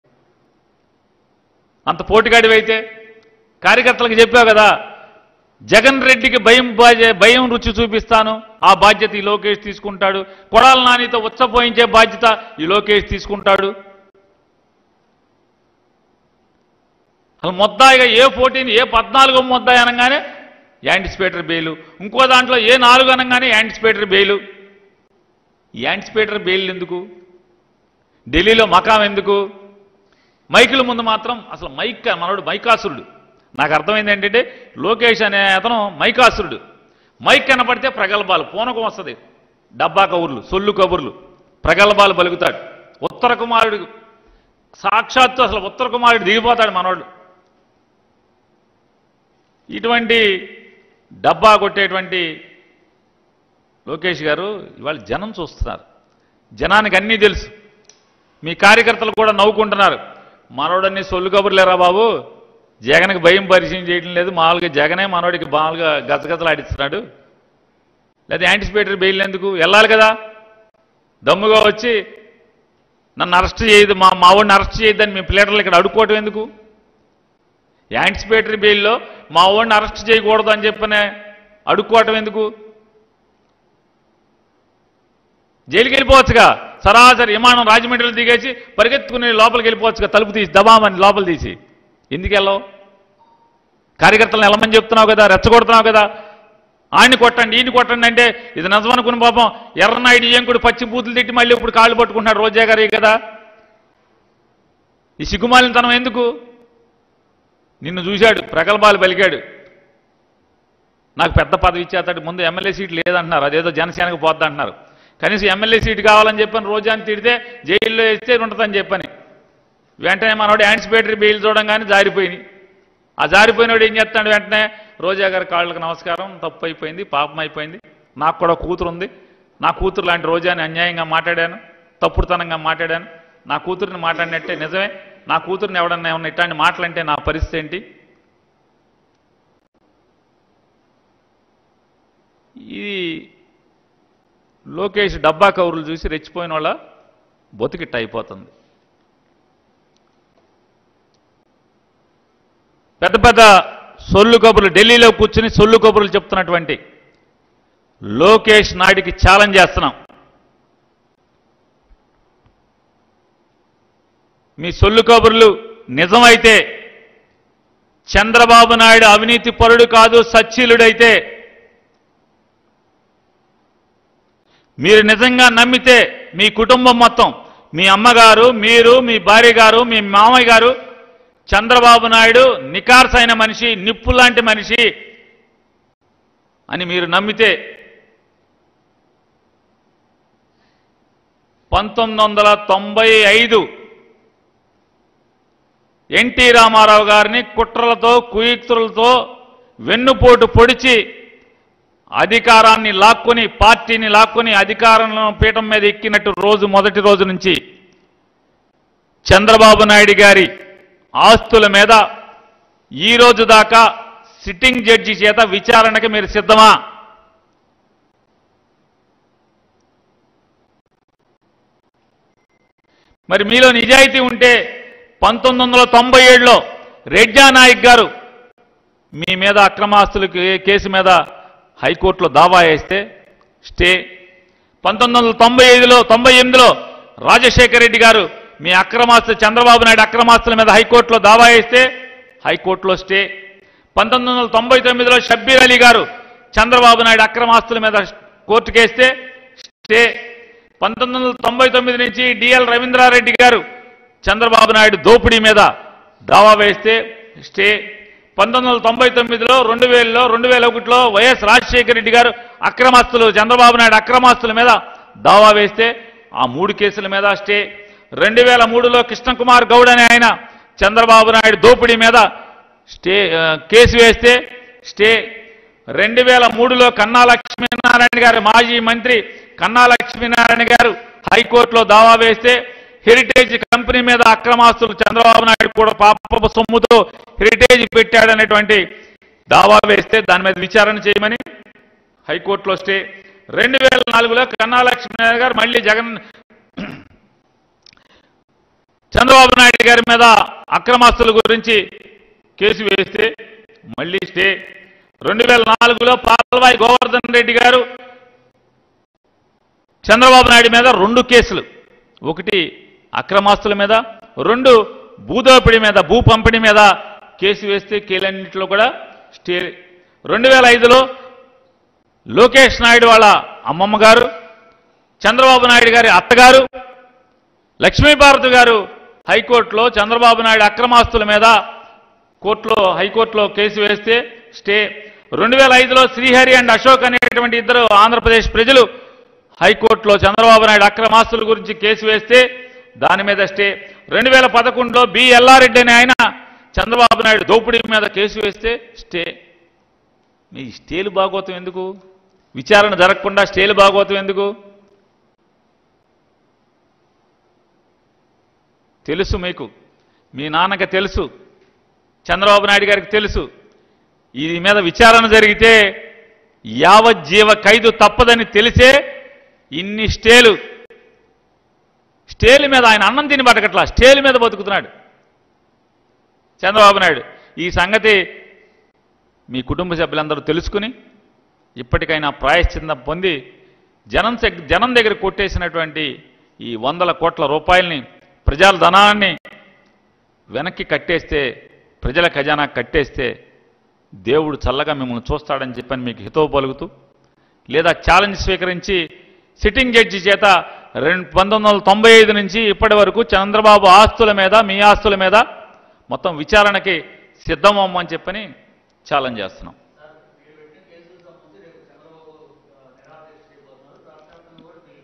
లోకేష్ కి పేర్ని నాని సవాల్..ఢిల్లీలో లోకేష్ సొల్లు కబుర్లు.. | Perni Nani | Mahaa News
LIVE🔴: పేర్ని నాని మీడియా సమావేశం | YCP Leader Perni Nani | Mahaa News